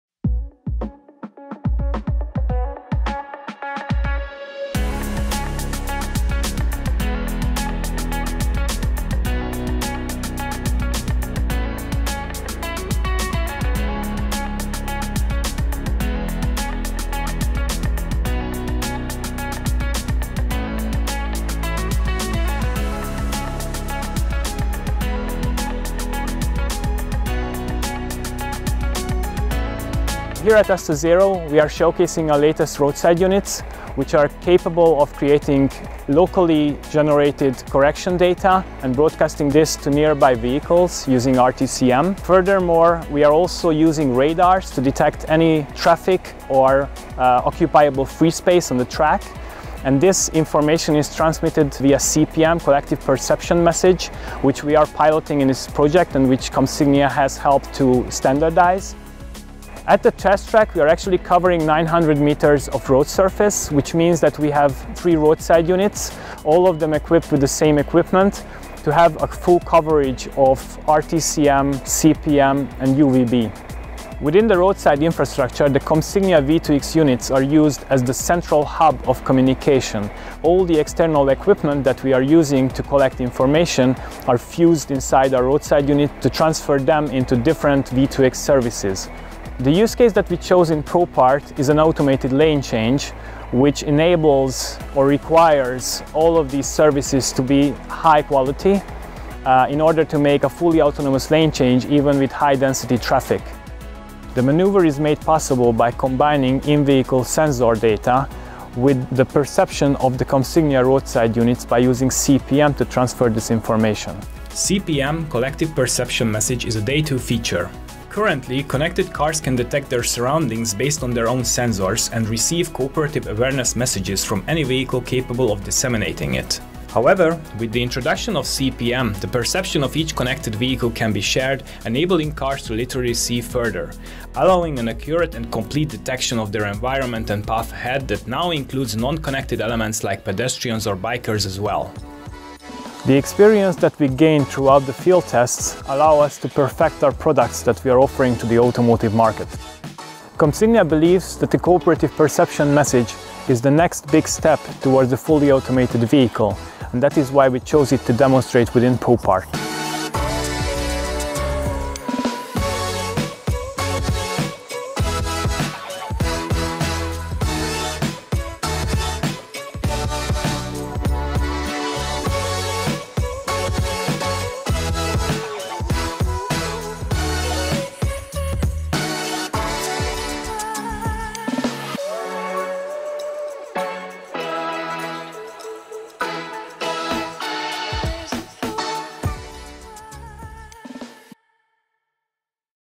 Meet Our Day 2 RSU and V2X Protocol Stack - A Case Study
CPM, Collective Perception Message is a Day 2 feature.

Currently, connected cars can detect their surroundings based on their own sensors, and receive cooperative awareness messages from any vehicle capable of disseminating it.

However, with the introduction of CPM, the perception of each connected vehicle can be shared, enabling cars to literally see further, allowing an accurate and complete detection of their environment and path ahead, that now includes non-connected elements, like pedestrians or bikers as well.